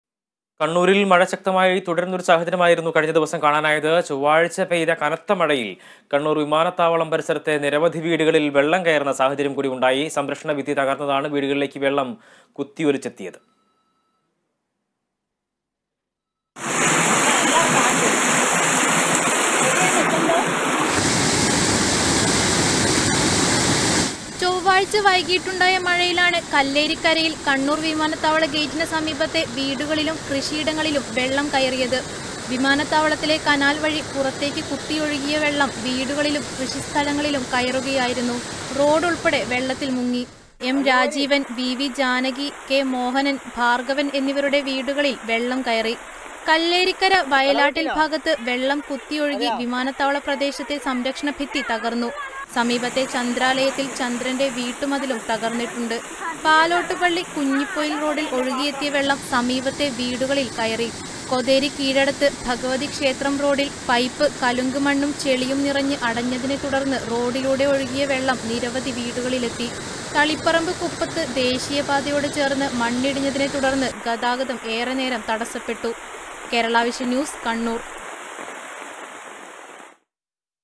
കനത്ത മഴയില്‍ കുത്തിയൊലിച്ച് വെള്ളം; കണ്ണൂരില്‍ വീടുകളില്‍ വെള്ളം കയറി | Kannur
കാലവര്‍ഷം ശക്തമായതിന് പിന്നാലെ കണ്ണൂരില്‍ മഴ ശക്തം. ചൊവ്വാഴ്ച പെയ്ത കനത്ത മഴയില്‍ കണ്ണൂര്‍ വിമാനത്താവളം പരിസരത്തെ നിരവധി വീടുകളില്‍ വെള്ളം കയറി. സംരക്ഷണഭിത്തി തകര്‍ന്നാണ് വീടുകളിലേക്ക് വെള്ളം കുത്തിയൊലിച്ചത്

About COA